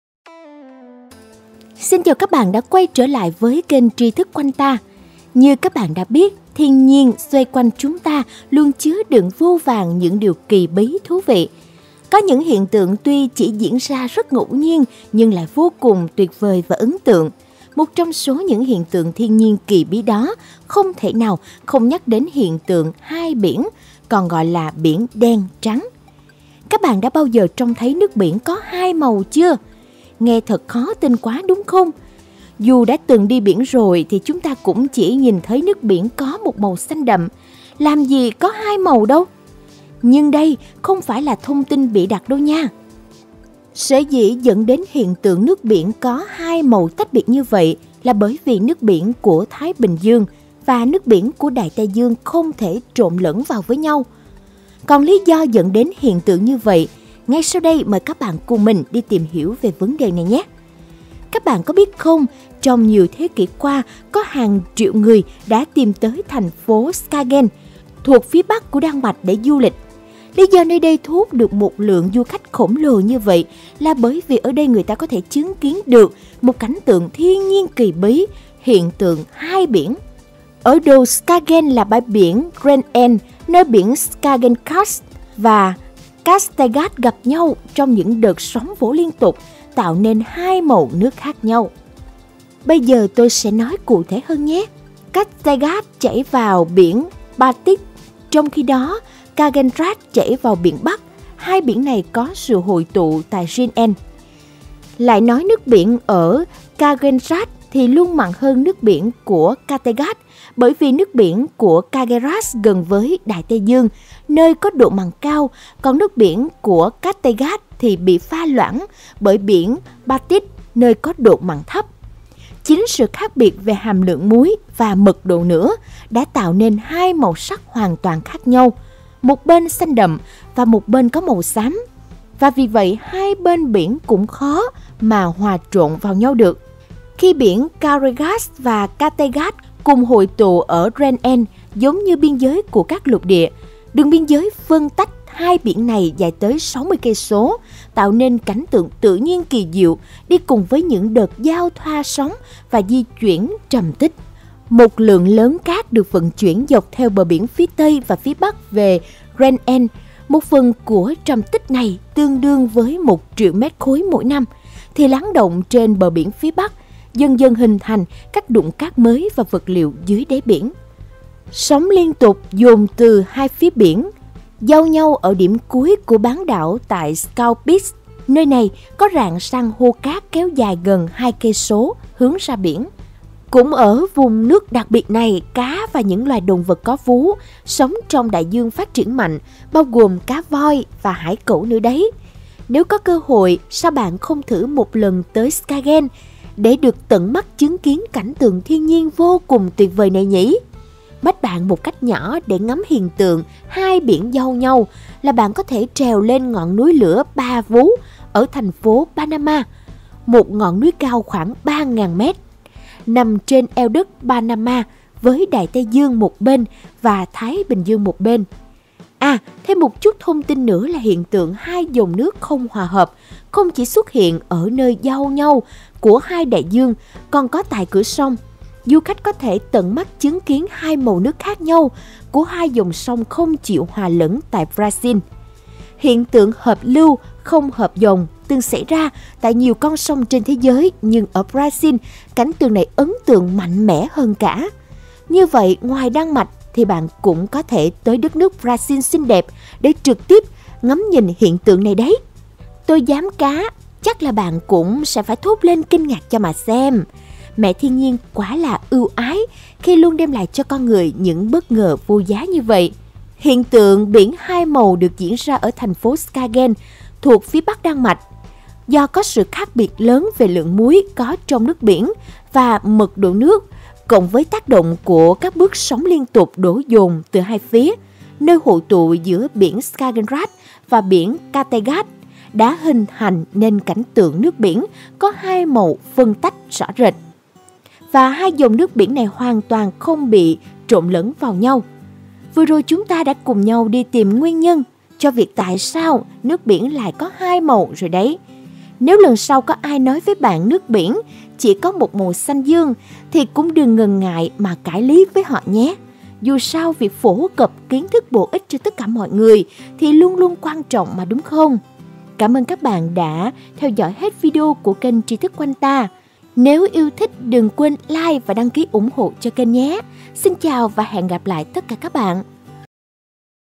Bí ẩn Nước Biển 2 màu một hiện tượng kỳ lạ rất thú vị | Tri Thức Quanh Ta #028
Bạn đã từng thấy hiện tượng nước biển 2 màu bao giờ chưa? Đây là hiện tượng thiên nhiên rất kỳ lạ và cũng khá thú vị đấy. Tri Thức Quanh Ta #028